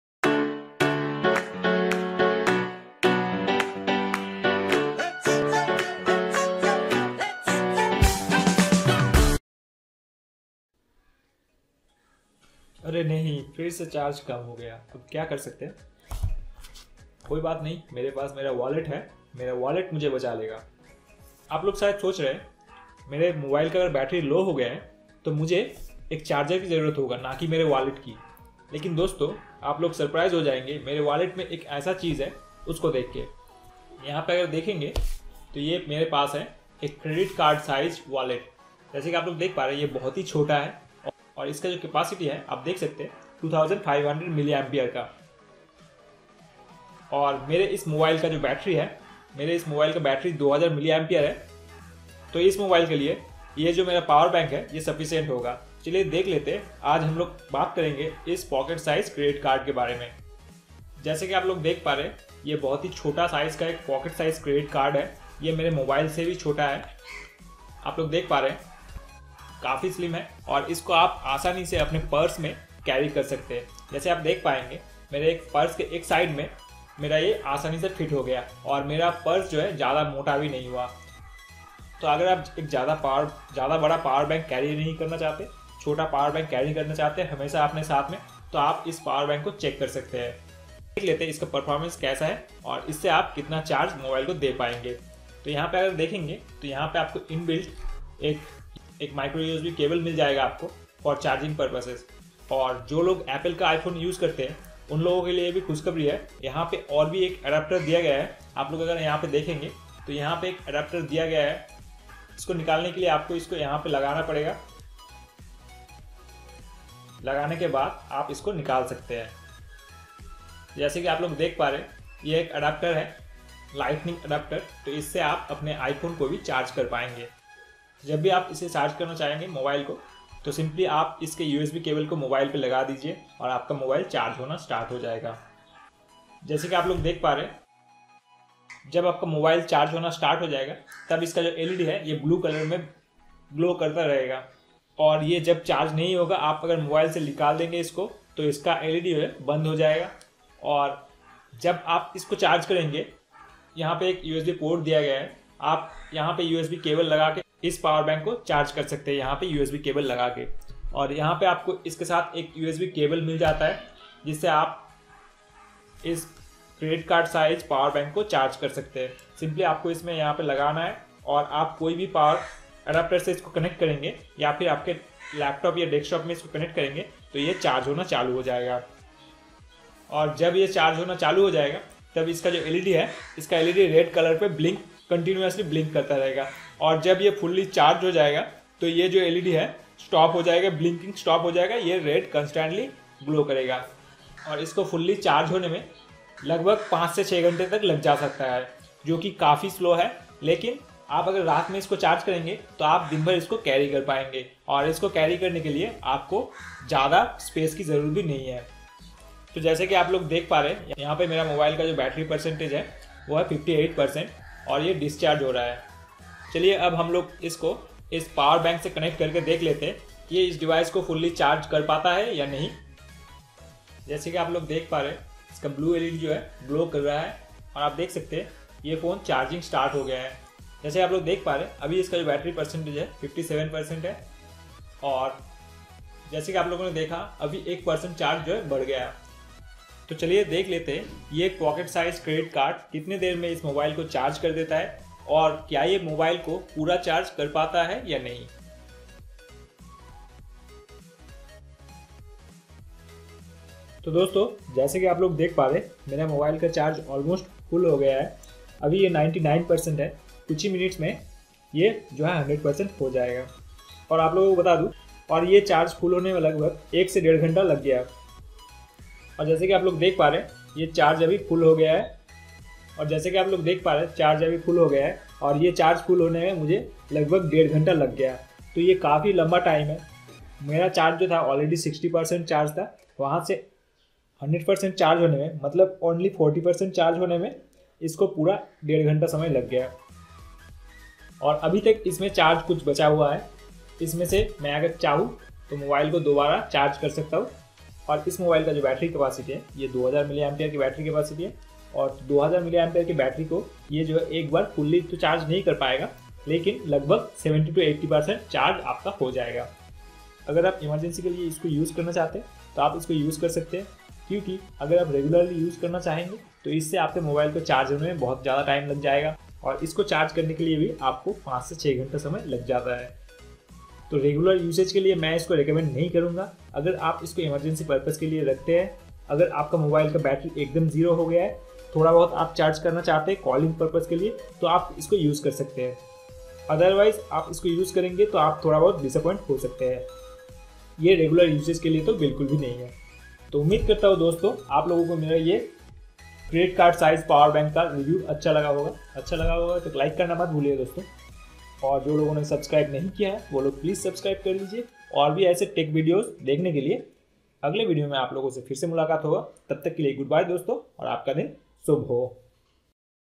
Credit Card Power Bank| Slim 2500 Mah Credit card power Bank | Android buddy
buy Samsung F23:

This is a very slim portable credit card size power bank.
Men/women Can easlily carry this power bank in their wallets.
But this power bank is pretty slow and takes too much time to get charged and Mobile charging time is also very high.
If you are looking slim stylish looking power bank then you can go for this power bank.